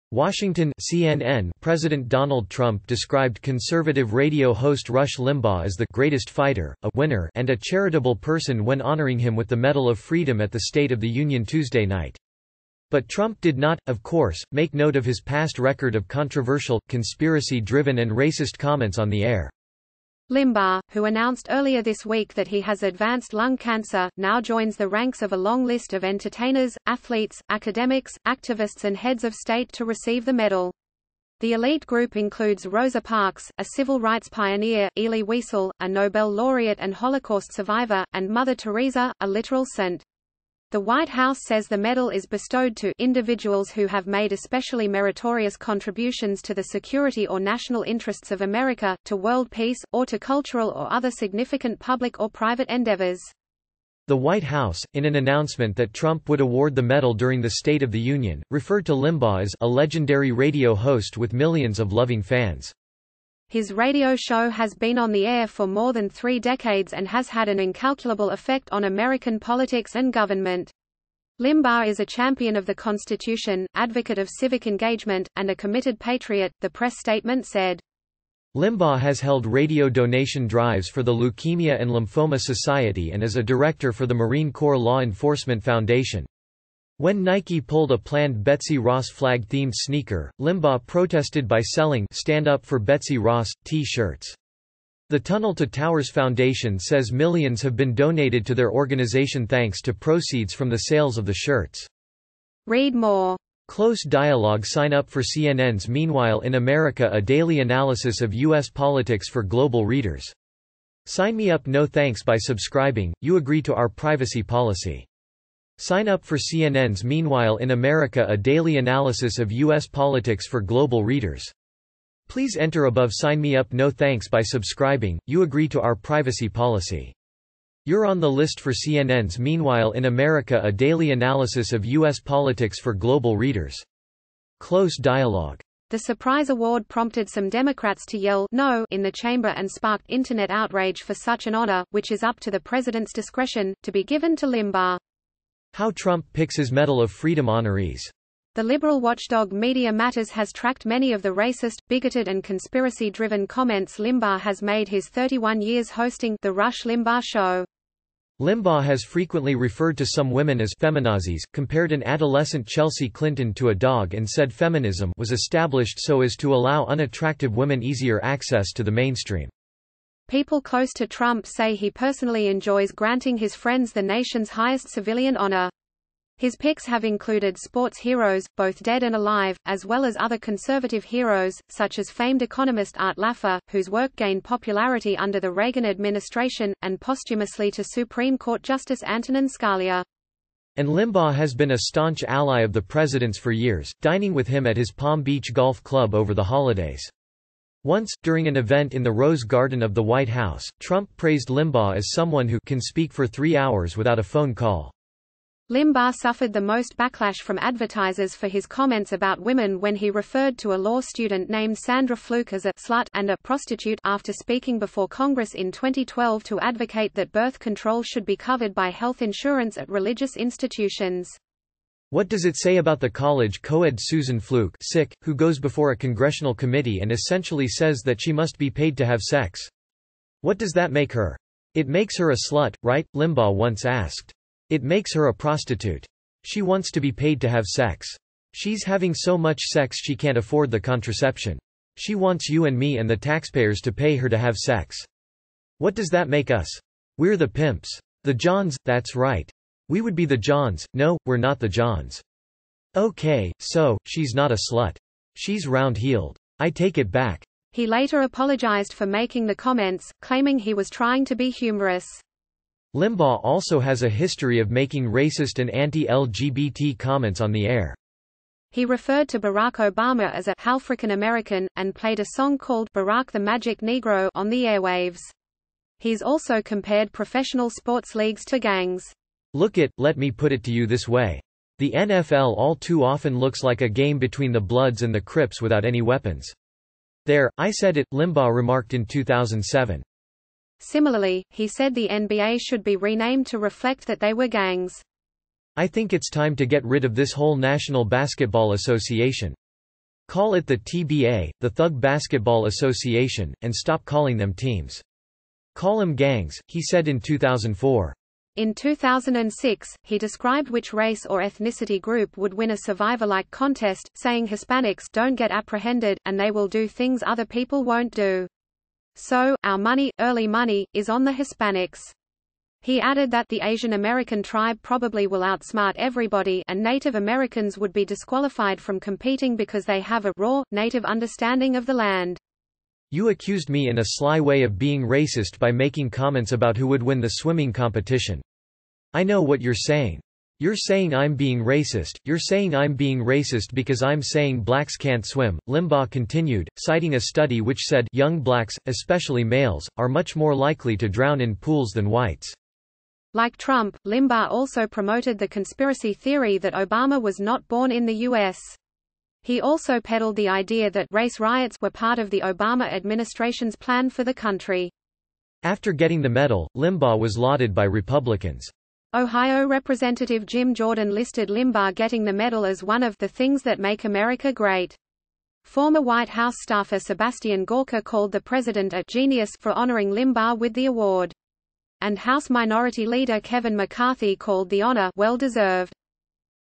Rush Limbaugh is a very different Medal of Freedom honoree
Washington (CNN) President Donald Trump described  conservative radio host Rush Limbaugh  as the "greatest fighter," a "winner" and a charitable person  when honoring him  with the Medal of Freedom at the State of the Union Tuesday night. But Trump did not, of course, make note of his past record of controversial, conspiracy-driven and racist comments on the air.